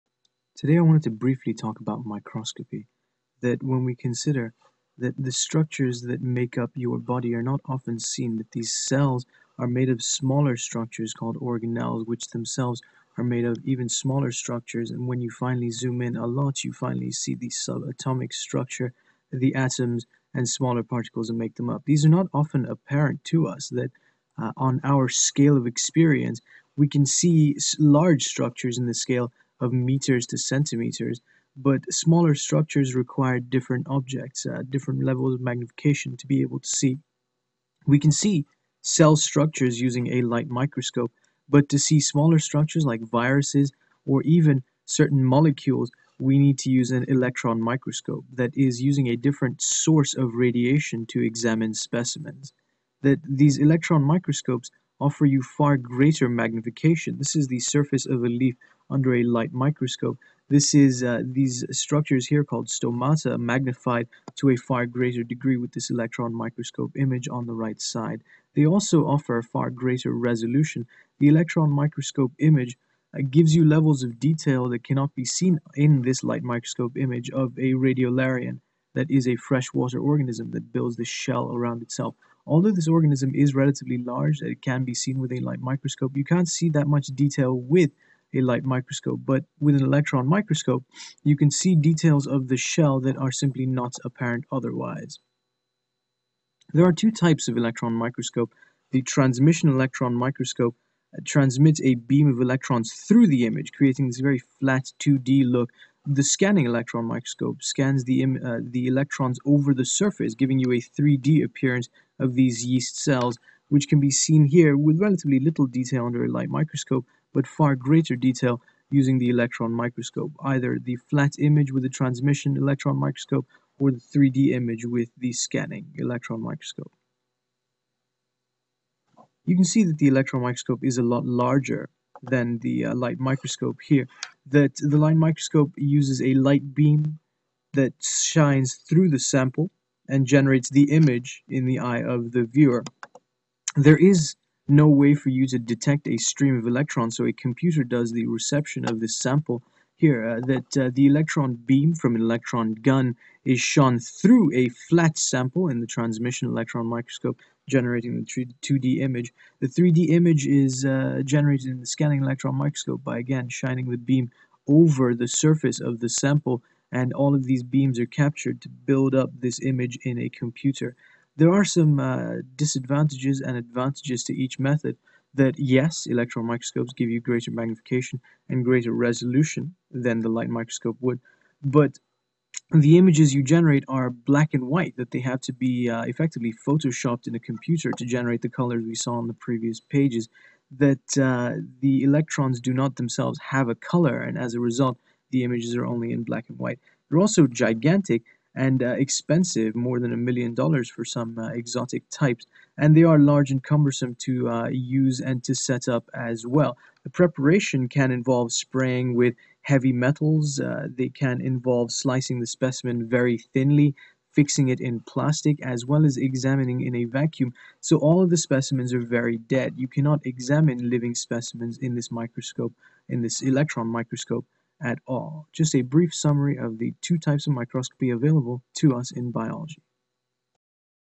Microscopy
Mr. Kulkarni explains the basic differences between light and electron microscopes, as used widely in Biology.